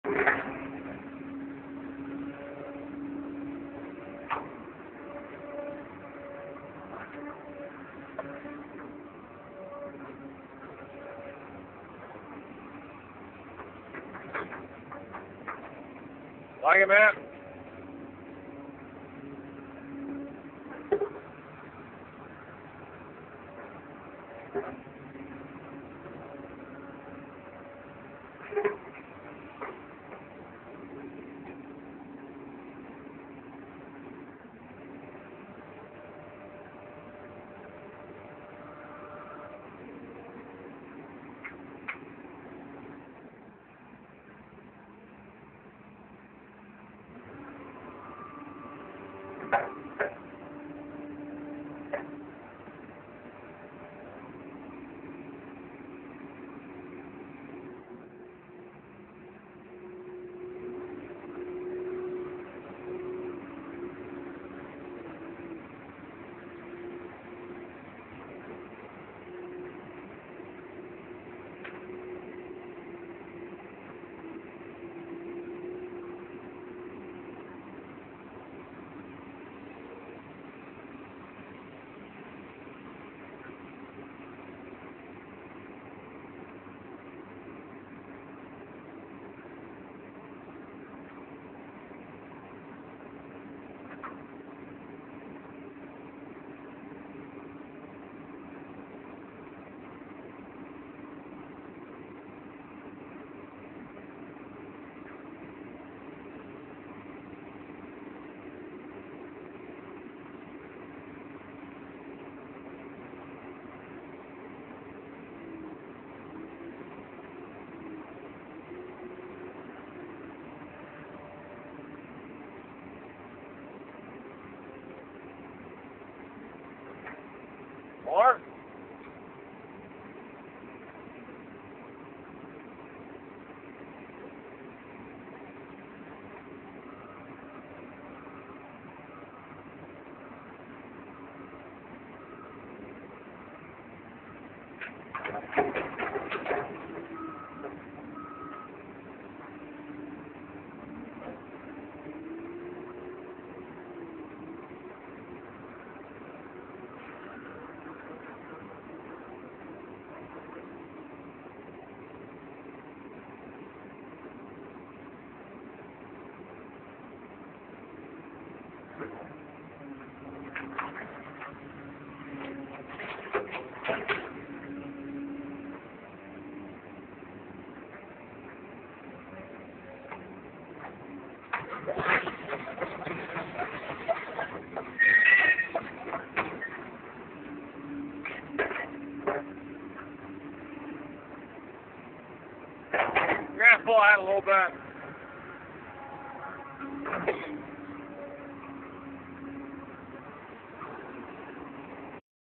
setting pipe with an excavator
We are laying 12" concrete storm pipe with an old, tired E200LC Cat excavator.  It is all layed on sand bedding, with sand backfill, which you will see a Cat 426 rubber tire backhoe dumping in the background.